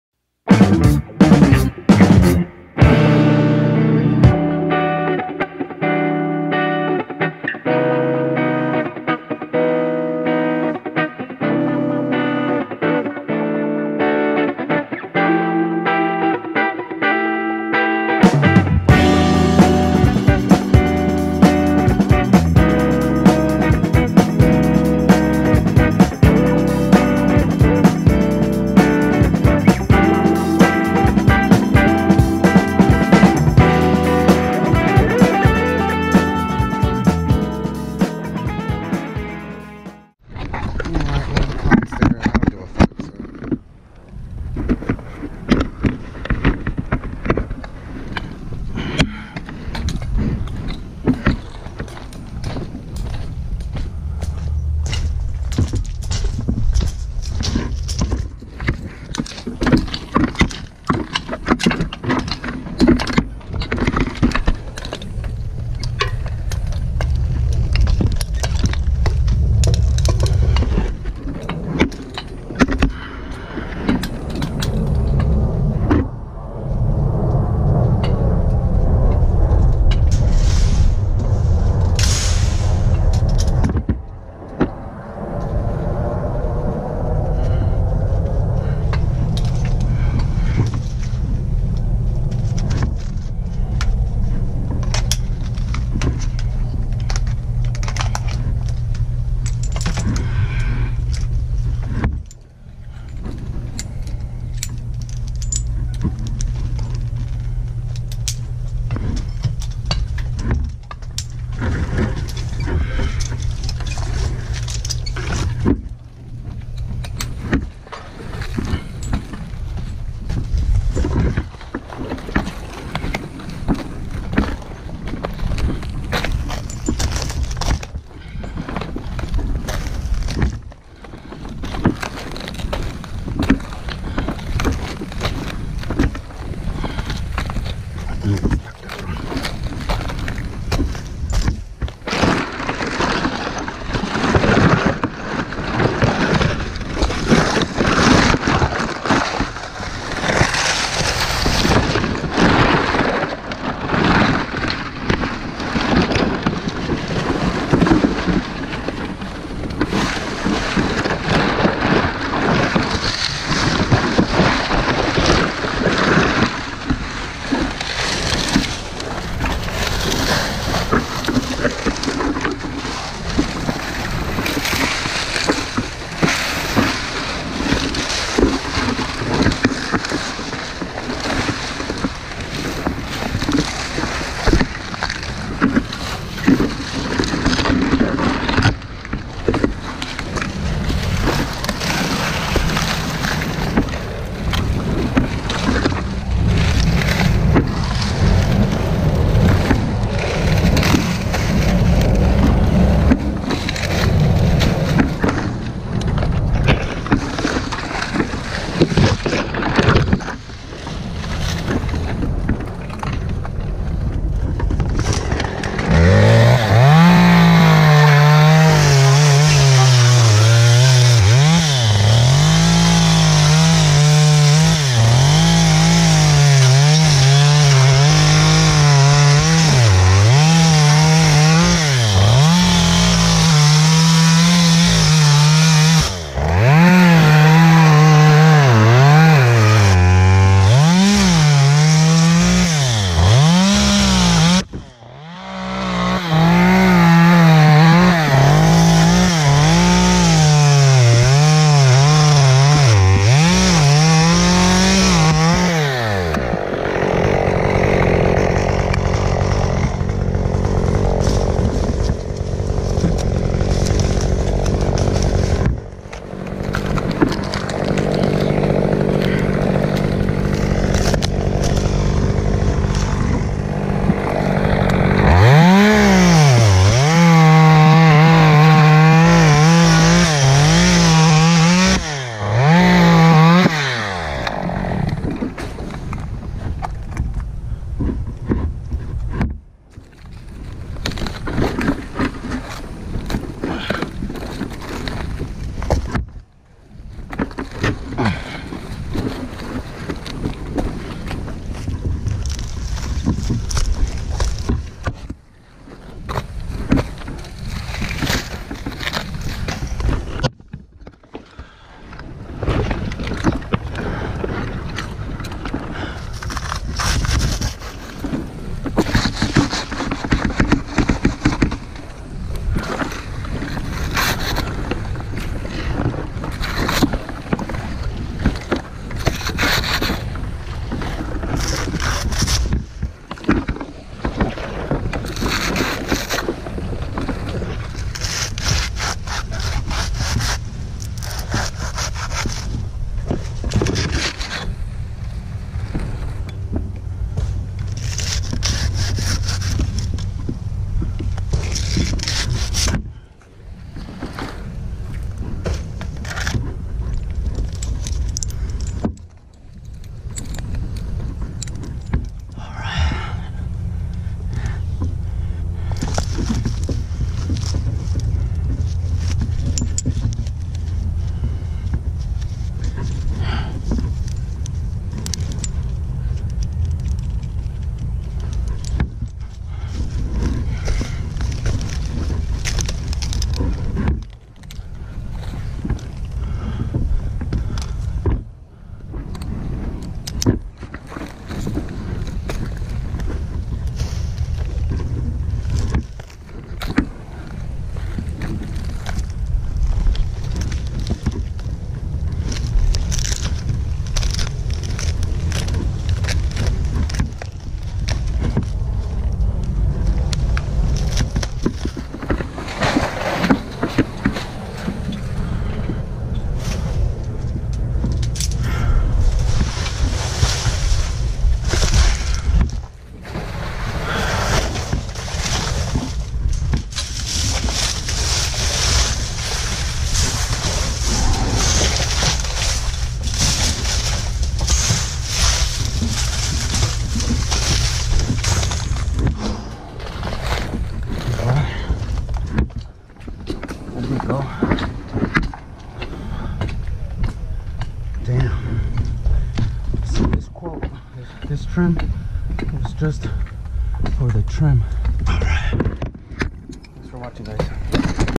1 PALM TREE, HOW LONG DOES IT TAKE TO TRIM?
in this video, i quoted the client for the trim (hurricane style) only. no debris will be hauled away

using the stihl 193t
zübat silky
petzl zigzag
2ft friction saver